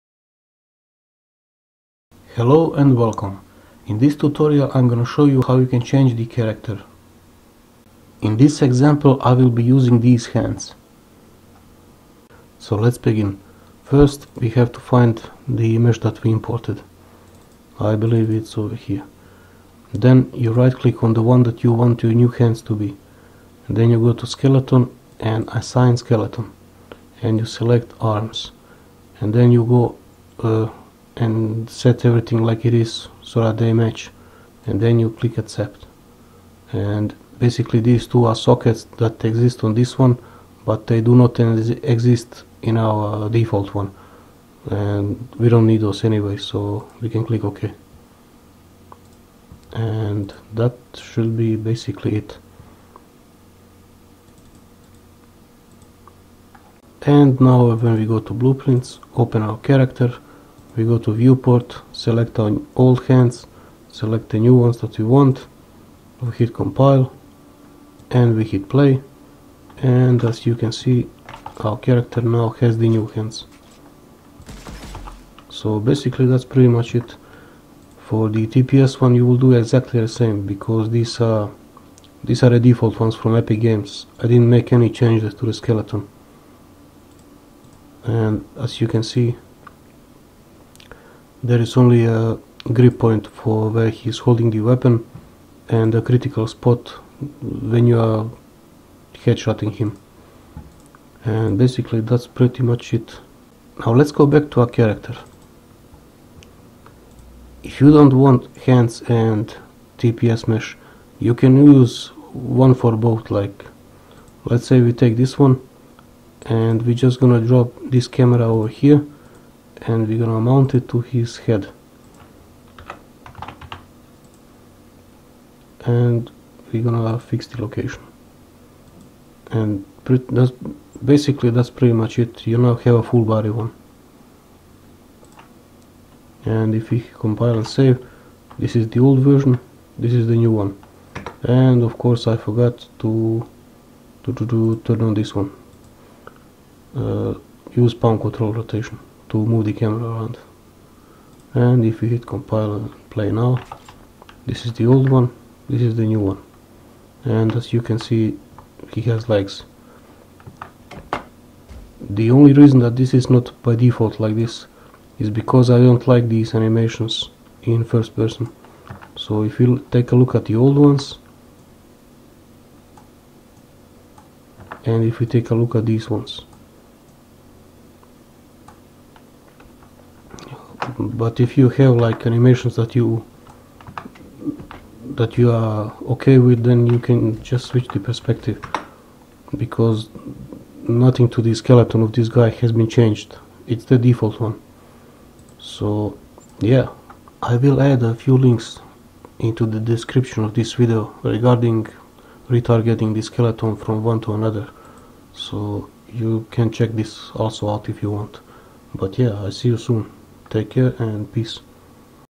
How to change the default mesh (FPS Game - Starter Kit)
A quick guide on how to change the default character mesh and make it have a full body instead of arms only.

For a more detailed and in depths explanation for retargeting (Different Skeletons) I highly recommend these links:

FPS Game - Starter Kit can be found on the UE4 Marketplace